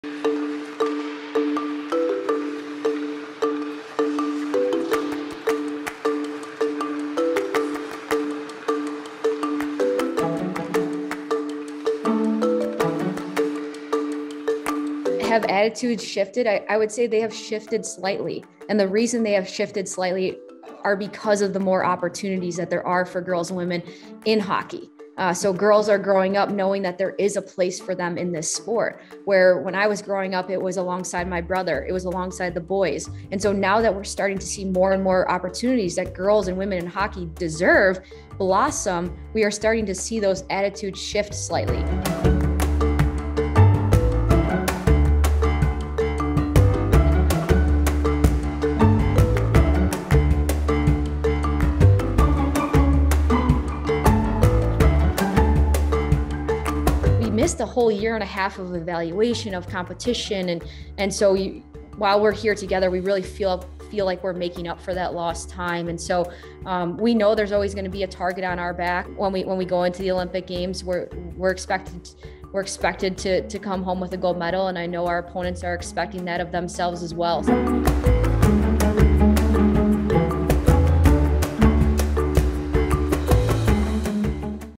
Kendall Coyne Schofield cuts new path for girls in hockey
American ice hockey star Kendall Coyne Schofield tells her story in a new book as she looks to earn another spot for the 2022 Winter Games in Beijing where Team USA hopes to defend their Olympic title.